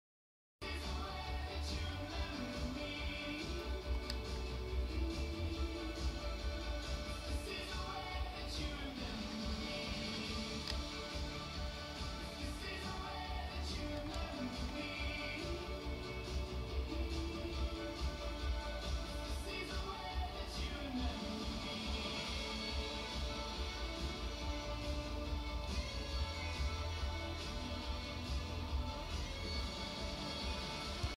Reported score without playing
The opponent reported score to their advantage (2:0) without showing up after 13min